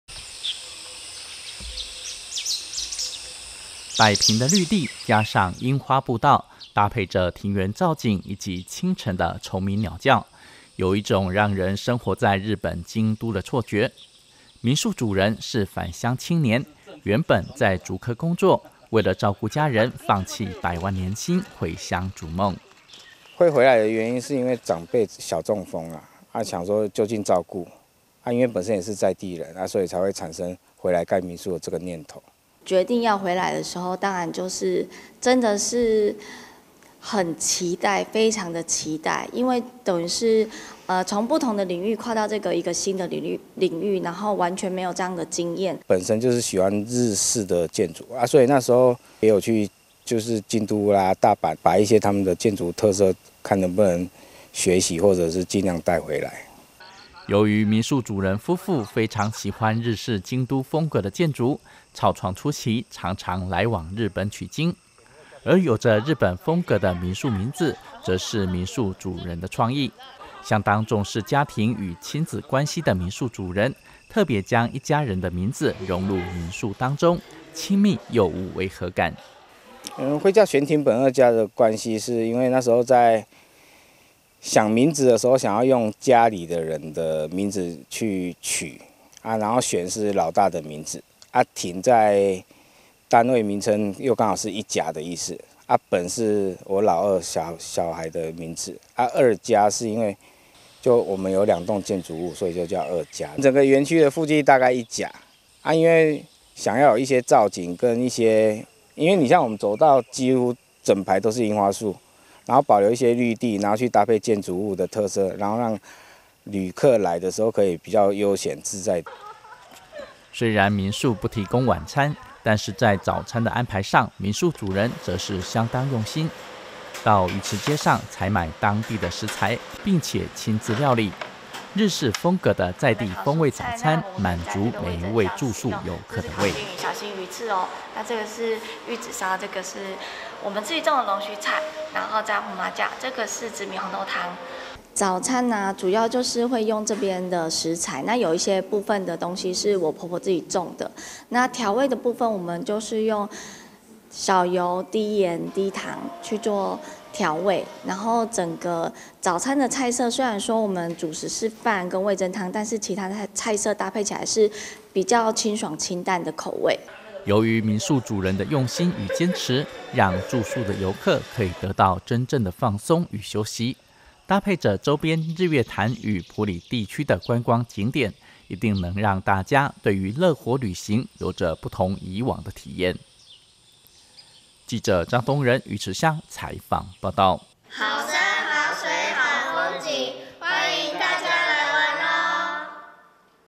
民宿京都風遊客驚艷 精選食材體驗魚池在地美好｜南投縣政新聞 2023.07.28
近年民宿、露營區話題相當火熱，位在魚池鄉的民宿「玄町本二家日式旅宿」有著濃濃的京都風格，常常成為網路社群熱搜焦點，民宿主人是竹科回歸的返鄉青年，與太太一手打造屬於自己心中的夢想城堡，並運用在地食材準備餐食，讓大家感受魚池在地的美好